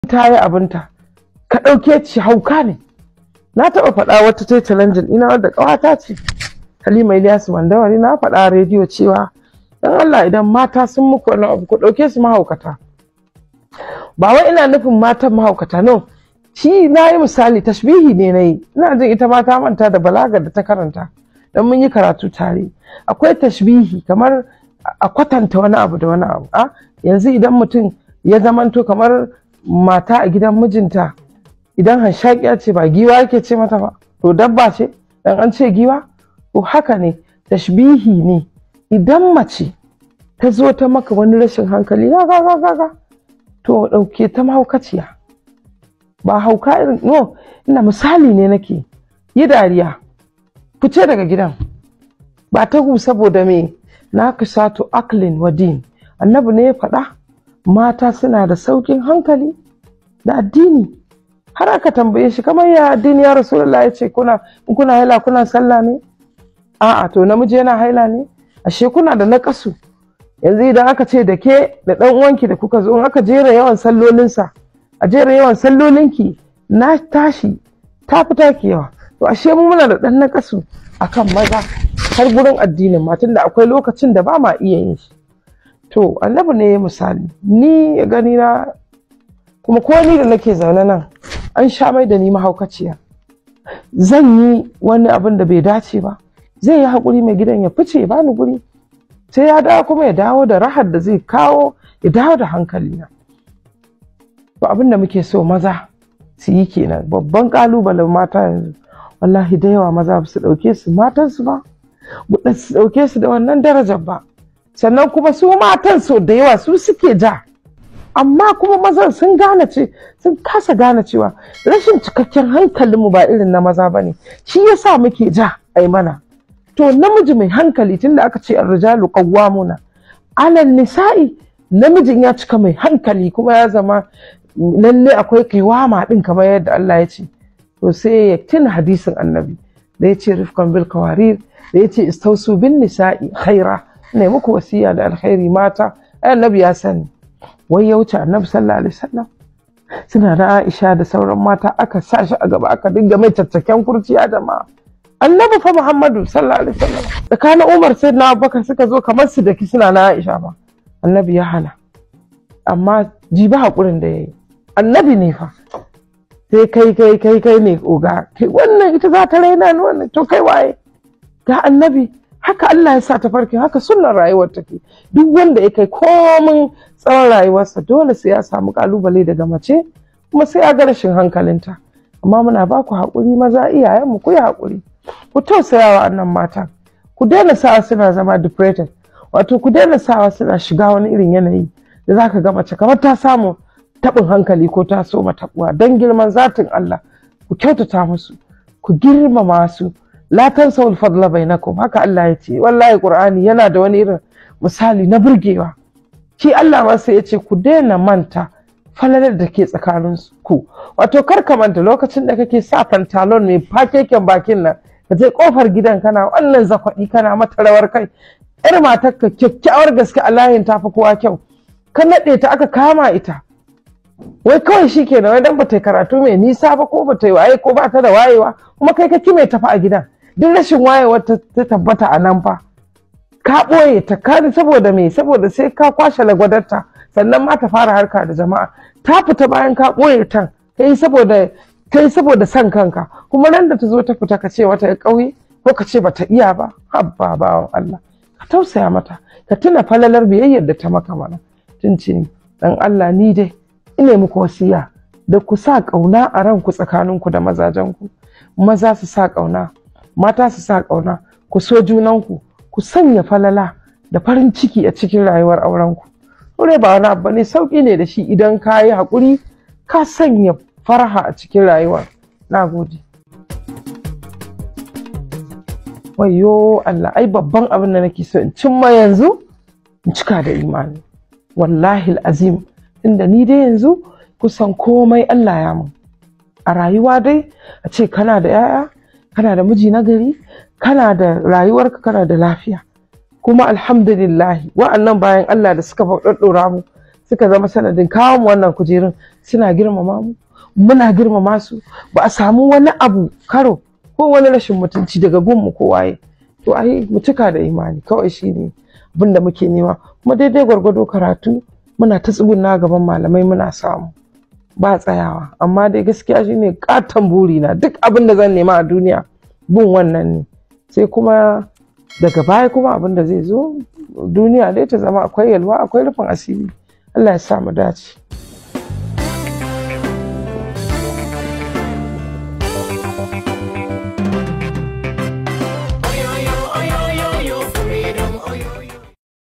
Episode 3: Wani lokaci idan mata sunyi muku wani abun ku dauke su mahaukata - Dr. Zahra'u
Kwamishiniyar mata da ƙananan yara ta Kano Dr. Zahra'u Muhammad Umar.